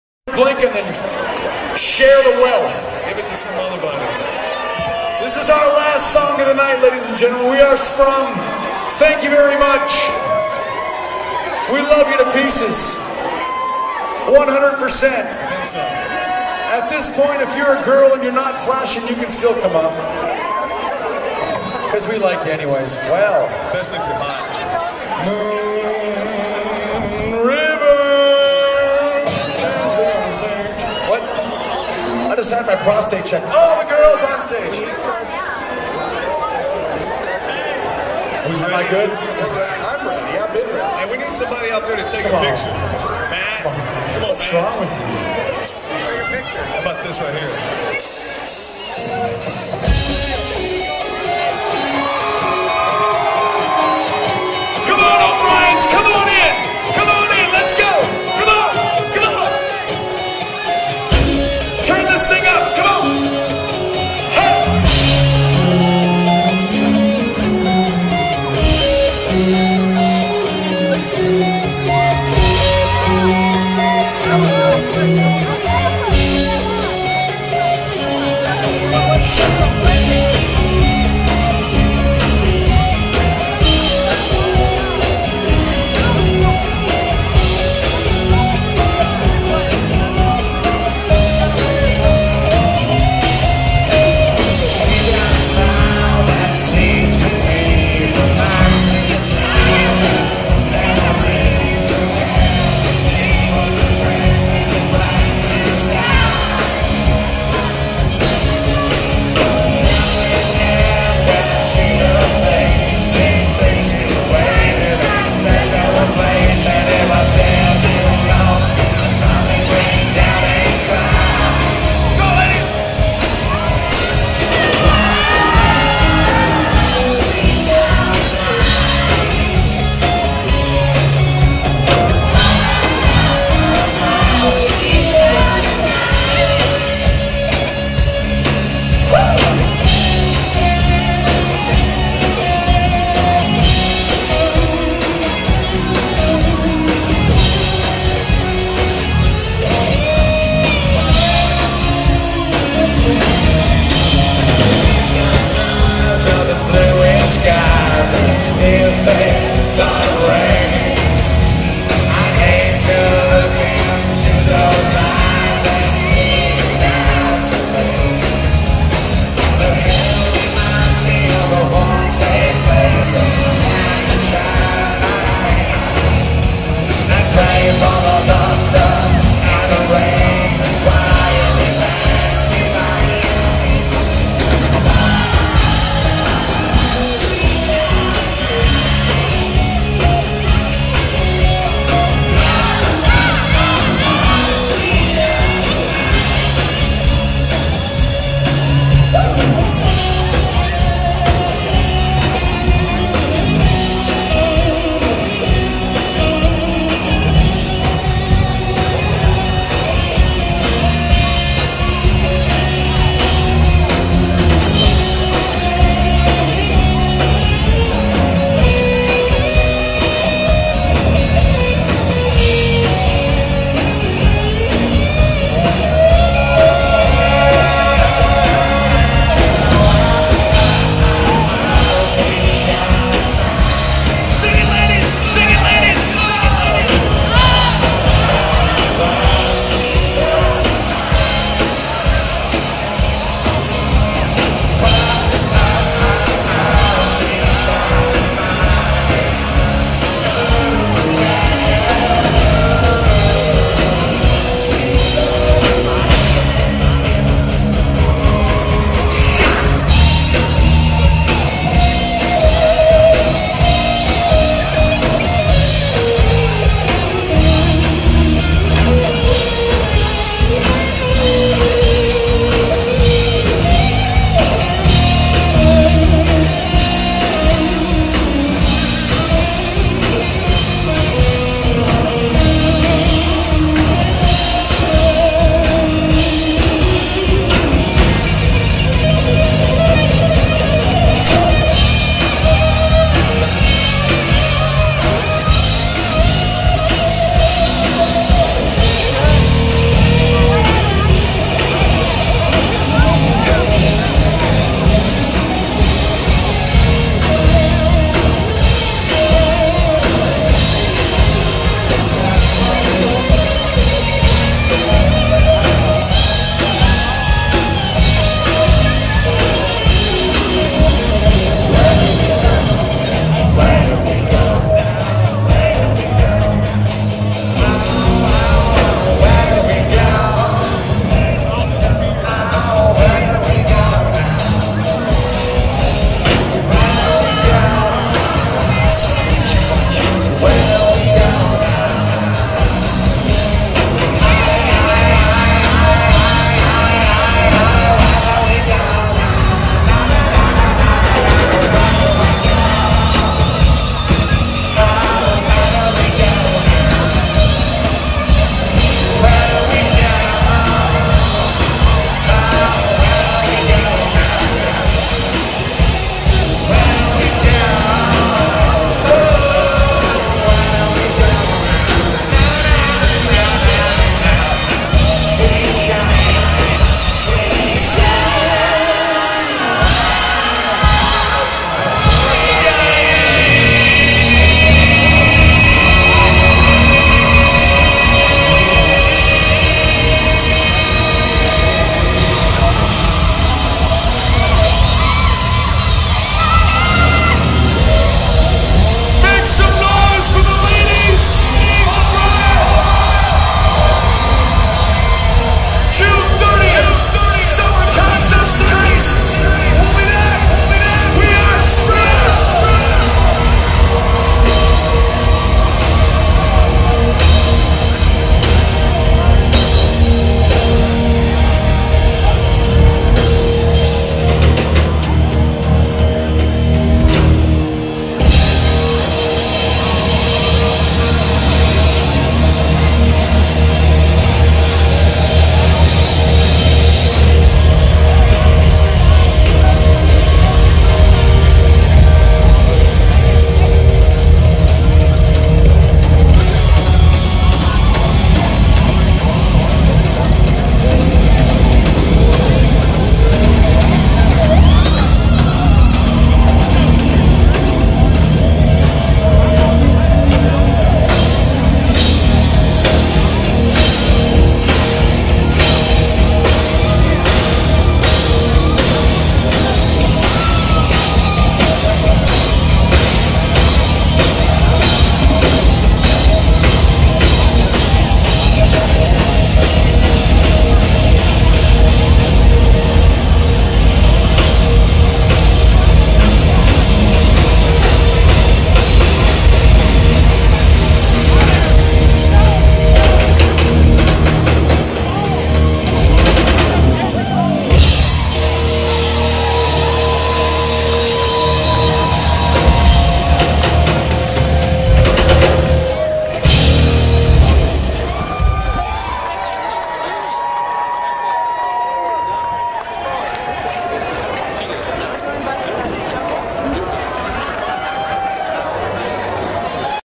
Sprung Saint Patrick's Day 2012 Clip 6
Sprung ending the night in the courtyard at Obriens Irish Pub Temple Tx on Saint Patrick's Day March 17, 2012.